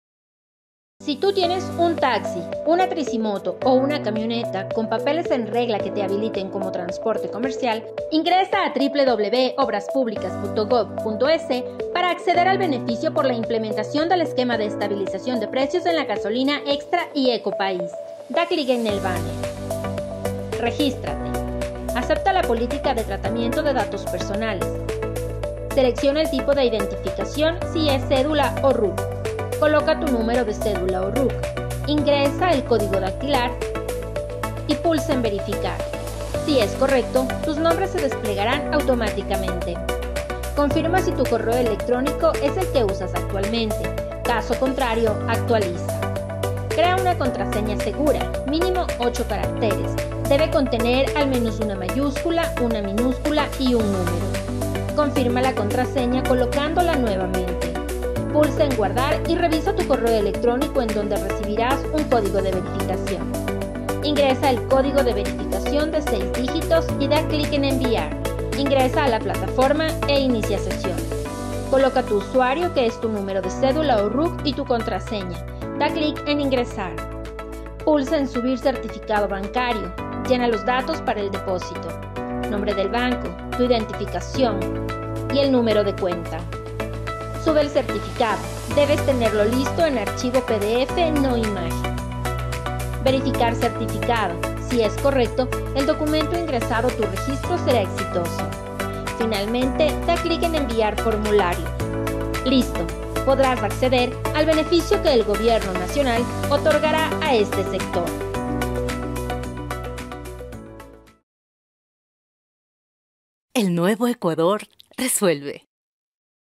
Video tutorial para registro de beneficiarios
Si tienes taxi, tricimoto o camioneta, con papeles en regla y habilitado como transporte comercial, regístrate en la plataforma que ha habilitado el Ministerio de Transporte y Obras Públicas y accede al beneficio que entregará el Gobierno Nacional como parte del mecanismo de protección a los usuarios de transporte por la implementación del esquema de estabilización de precios en la gasolina extra y ecopaís.